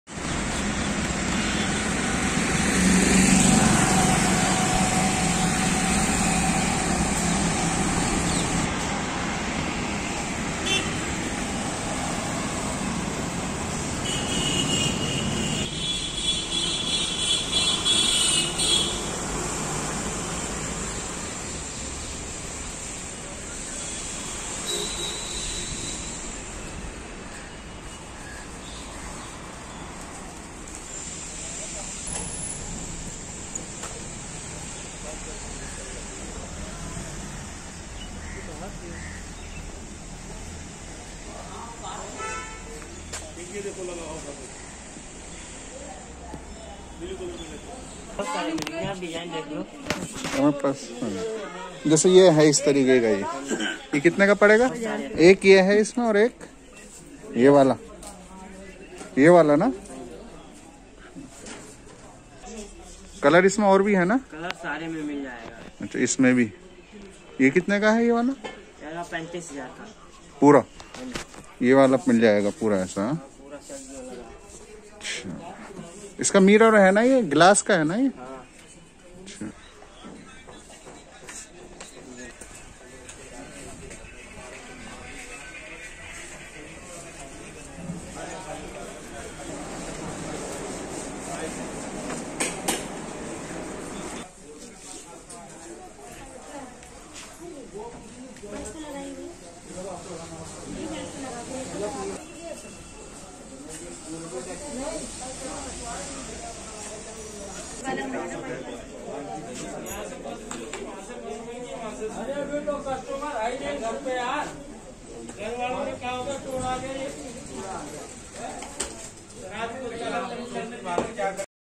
Panchkuian Road Furniture Market Delhi - Panchkuian Furniture Market Reviews
PANCHKUIAN FURNITURE MARKET

A one-stop destination for all your furniture requirements, whether it's elegant wooden finished beds, or reclining chairs, sofa cum beds, or office furniture, they literally have it all!
We're all pretty much done with online shopping now, and since the pandemic will remain to exist till whenever the vaccine is out, many people are now venturing out and heading to the markets like the normal days. If you're looking out for affordable and amazing furniture then we recommend you head to Panchkuian Furniture Market.

Established after the India-Pakistan division, many Punjabi's from the neighbouring country came and settled around the market area initially. In the beginning, the market was known to sell a variety of items but ever since 1960, many carpenters and artists started settling in that area and starting selling furniture and home decor. And now, the demand has increased so much that the market is only catering to amazing furniture and home decor items.

NEW??

Topic Cover -
Panchkuian Furniture Market Delhi | The most famous furniture MARKET since 1960 @Super Low Price